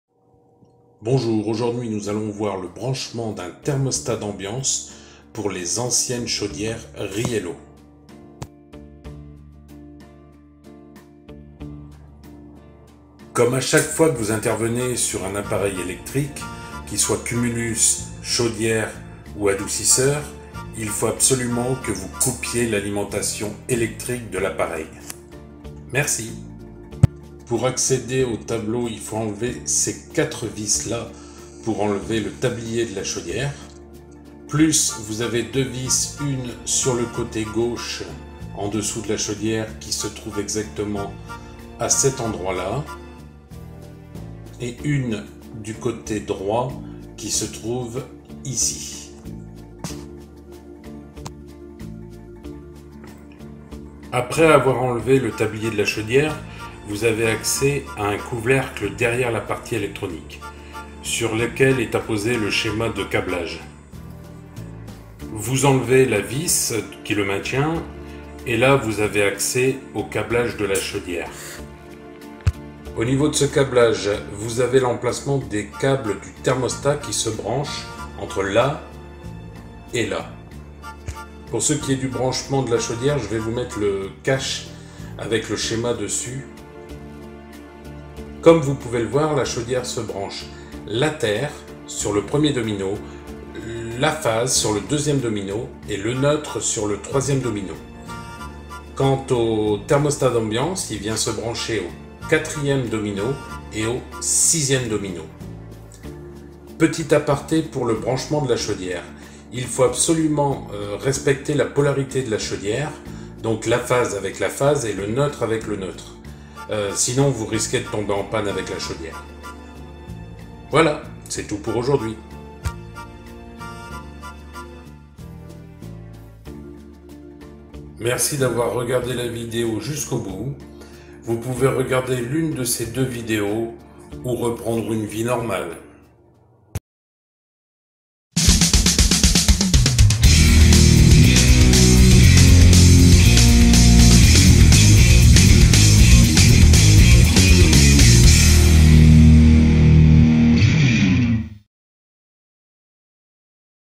Comment brancher un thermostat d'ambiance sur une chaudière Riello ?
Dans ce tutos, je vous dit comment et où brancher un thermostat d'ambiance sur une chaudière Riello.

Vidéo n°231